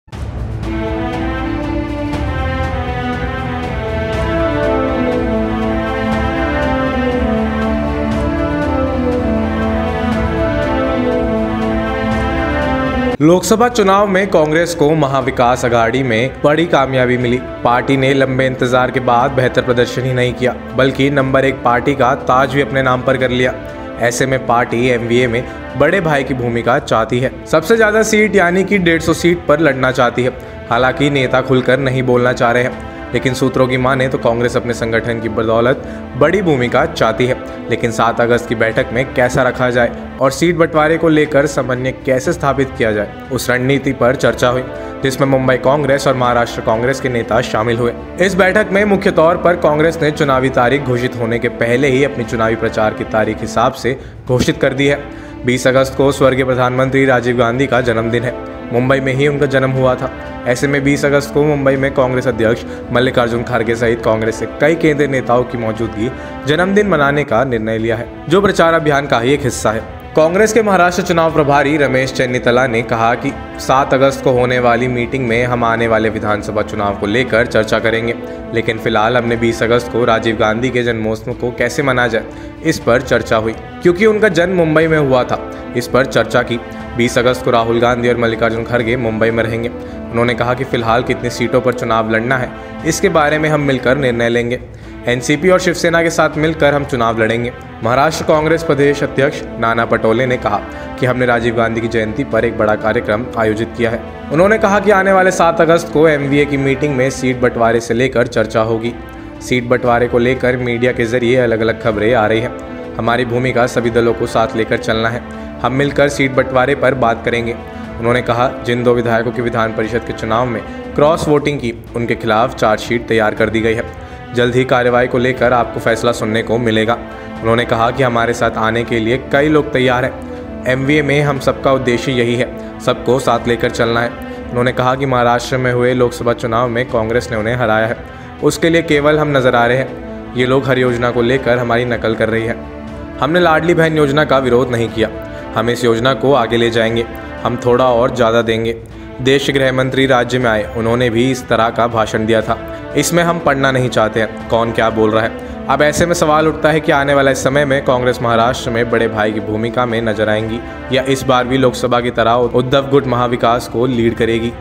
महाराष्ट्र में कांग्रेस को कितनी सीटें मिली ? CONGRESS | MAHARASHTRA
In the Lok Sabha elections, Congress got a big success in Mahavikas Aghadi. After a long wait, the party not only performed better but also won the crown of the number one party. In such a situation, the party wants the role of elder brother in MVA.